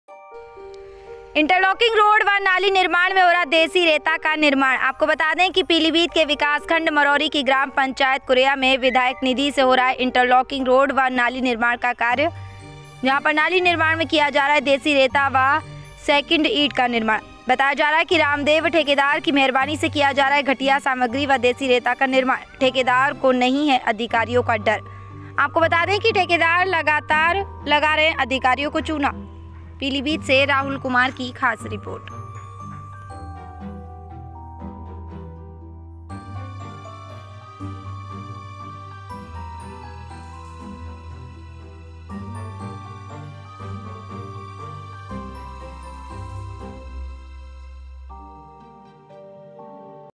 इंटरलॉकिंग रोड व नाली निर्माण में हो रहा देसी रेता का निर्माण
उत्तर प्रदेश पीलीभीत

पीलीभीत के  विकास खंड मरौरी की ग्राम पंचायत कुरैया  में विधायक निधि से हो रहा इंटरलॉकिंग रोड व नाली निर्माण का कार्य  जहां पर नाली निर्माण
में किया जा रहा देसी रेता वा सेकेंड ईंट का निर्माण राम देव ठेकेदार की मेहरबानी से किया जा रहा  घटिया सामग्री व देसी रेता का निर्माण   ठेकेदार को नही है अधिकारियो का डर  ठेकेदार लगातार लगा रहे अधिकारियों को चुना

पीलीभीत से राहुल कुमार की रिपोर्ट